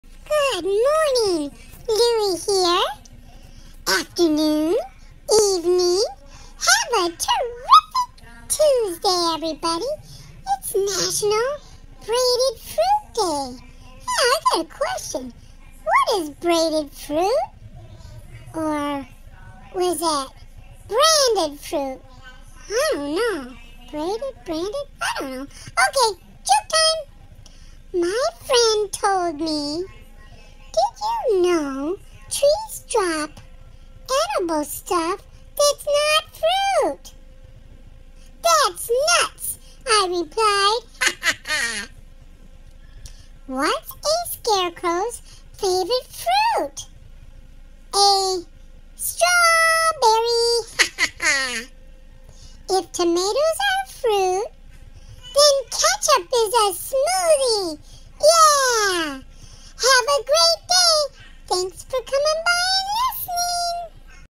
Good Morning 🌞/Afternoon/Evening...Louie Here! National Brandied Fruit Day!
If tomatoes are a fruit
Then ketchup is a smoothie.

National Brandied Fruit Day
Date When Celebrated : Always October 20 th
National Brandied Fruit Day celebrates sweet tasting fruit, soaked and marinated in brandy. This is a day that you have most certainly booked on your calendar, in eager anticipation. Well, it has finally arrived.
Brandied fruit...now that's cause for a celebration! As a matter of fact, I'll drink to that.
Enjoy a little brandied fruit on this day. Do so, with family and friends.
We hope you have a blissful National Brandied Fruit Day.
History and Origin of National Brandied Fruit Day:
Our research did not find the creator, or the origin of this day. Was the originator of this day a little tipsy when he or she created it? We'll let you know when we find them.
Like so many other food related special says, this is often referred to as a "National" day. We did not find any documentation confirming this to be a "National" day. We found no congressional records or presidential proclamation.
Love 💕 & Hugs 🤗